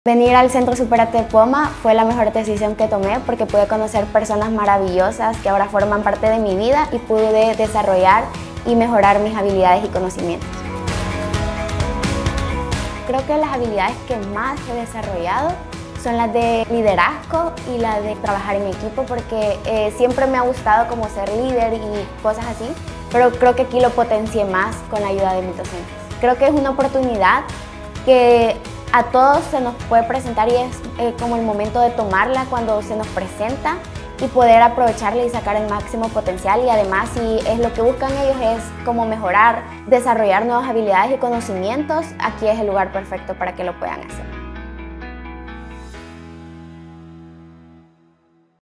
Iris Salguero | ¡Desarrolla tu máximo potencial! 👩‍🎓👨‍🎓
ConvocatoriaAbierta | ¡Desarrolla tu máximo potencial! 👩‍🎓👨‍🎓

Iris Salguero nos cuenta porqué ingresar a ¡Supérate! fue su mejor decisión. Ahora, como estudiante de tercer año en nuestro Centro ¡Supérate! Fundación Poma, nos cuenta cómo ha desarrollado habilidades de liderazgo y trabajo en equipo que le han permitido sobresalir. 🌟

🌎 Bienestar es progreso social.